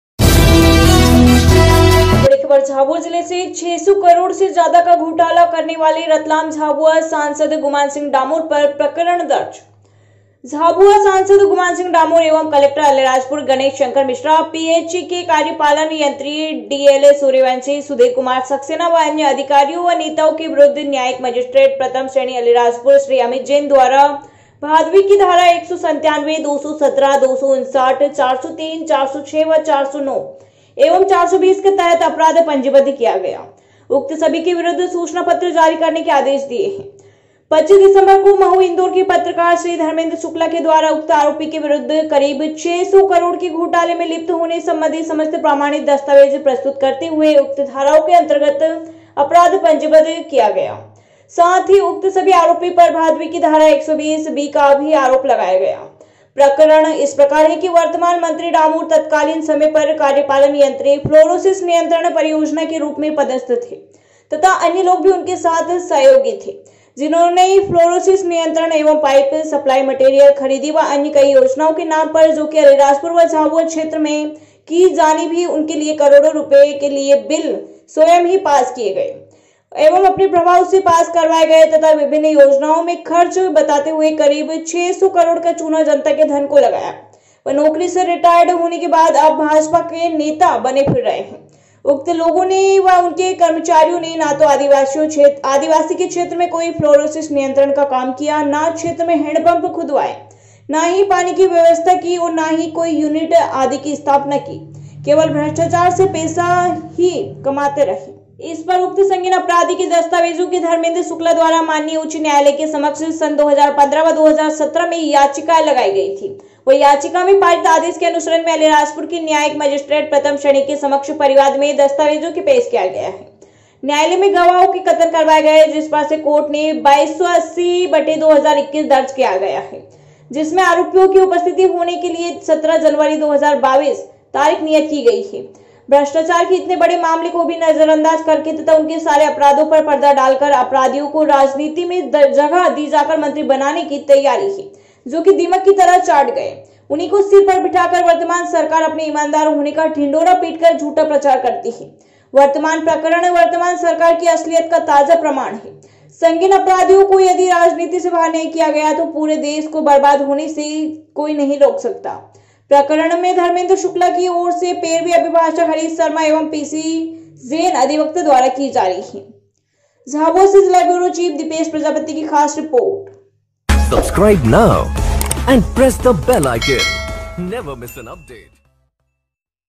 झाबुआ रतलाम सांसद गुमान सिंह डामोर 600 करोड़ से ज्यादा का घोटाला करने पर प्रकरण दर्ज हुआ
झाबुआ से जिला ब्यूरो चीफ दीपेश प्रजापति की रिपोर्ट
झाबुआ
600 करोड़ से ज्यादा का घोटाला करने वाले रतलाम झाबुआ सांसद गुमान सिंह डामोर पर प्रकरण दर्ज

रतलाम/झाबुआ ।  सांसद गुमानसिंह डामोर एवं कलेक्टर अलीराजपुर गणेशशंकर मिश्रा पी एच ० ई ० के कार्यपालन यंत्री डी .एल सूर्यवंशी, सुधीरकुमार सक्सेना व अन्य अधिकारियों व नेताओं के विरुद्ध न्यायिक मजिस्ट्रेट प्रथम श्रेणी ,अलीराजपुर श्री अमित जैन द्वारा भादवि की धारा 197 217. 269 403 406 , 409 एवं 420 के तहत अपराध पंजीबद्ध किया है तथा उक्त सभी के विरुद्ध सूचना पत्र जारी करने के आदेश दिये है । इन्दौर दिनांक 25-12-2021 महू इन्दौर के पत्रकार श्री धर्मेन्द्र शुक्ला के द्वारा उक्त आरोपी के विरुद्ध करीब छै सो करोड के घोटाले में लिप्त होने संबंधी समस्त प्रमाणित दस्तावेज प्रस्तुत करते हुये उक्त धाराओं के अन्तर्गत अपराध पंजीबद्ध किया जाकर उनके विरूद्ध है। साथ ही उक्त सभी आरोपी पर भादवि की धारा 120 बी का आरोप भी लगाया गया है । प्रकरण इस प्रकार है कि वर्तमान मंत्री डामोर तत्कालिन समय पर कार्यपालन यंत्री पलोरोसिस नियंत्रण परियोजना के रूप में पदस्थ था तथा अन्य लोग भी उसके साथ सहयोगी थे। जिन्होंने फ्लोरोसिस नियंत्रण एवं पाईप सप्लाई मटेरियल खरीदी व अन्य कई योजनाओं के नाम पर जो कि अलीराजपुर और झाबुआ क्षेत्र में की जानी भी उनके लिये करोड़ों रुपये के बिल स्वयं ही पास किये गये और अपने प्रभाव से पास करवाये गये तथा विभिन्न योजनाओं में खर्च बताते हुये करीब छाछ सो करोड़ का चुना जनता के धन को लगाया और नौकरी से रिटायर्ड होने के बाद अब भाजपा का नेता बना फिर रहा है । उक्त लोगों ने और उनके कर्मचारियों ने ना तो आदिवासी क्षेत्र में कोई फ्लोरोसिस नियंत्रण का काम किया ना क्षेत्र मे हेड पप्प खुदवाये ना पानी की व्यवस्था की ना ही कोई युनिट आदि की स्थापना की केवल भ्रष्टाचार से पैसा ही बनाते रहे ।। इस पर उक्त संगीन अपराधी के दस्तावेजों के धर्मेन्द्र शुक्ला द्वारा माननीय उच्च न्यायालय के समक्ष सन 2015 एवं 2017 में याचिकाएं लगाई गई थी और याचिका में पारित आदेश के अनुशरण मे अलीराजपुर की न्यायिक मजिस्ट्रेट प्रथम श्रेणी के समक्ष परिवाद मय दस्तावेजों के पेश किया है और न्यायालय में गवाहों के कथन करवाये है। जिस पर से कोर्ट ने 2280 / 2021 दर्ज किया गया है जिसमे आरोपियों की उपस्थित होने के लिये दिनांक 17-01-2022 तारीख नियत की गई है । भ्रष्टाचार के इतने बड़े मामले को भी नजर अंदाज करके तथा उनके सारे अपराधों पर पर्दा डालकर अपराधियों को राजनीती में जगह दी जाकर मंत्री बनाने की तैयारी है जो दीमक की तरह चाट गये उन्हीं को सिर पर बिठा कर वर्तमान सरकार अपने ईमानदार होने का दिढोरा पीटकर झूठा प्रचार करती है । वर्तमान प्रकरण वर्तमान सरकार की असलियत का ताजा प्रमाण हैं । संगीन अपराधियों को यदि राजनिती से बाहर नहीं किया गया तो पुरे देश को नष्ट होने में समय नहीं लगेगा । प्रकरण में धर्मेन्द्र शुक्ला की और से पैरवी अभिभाषक हरीश शर्मा एवं पीसी जैन अधिवक्ता द्वारा की जा रही है ।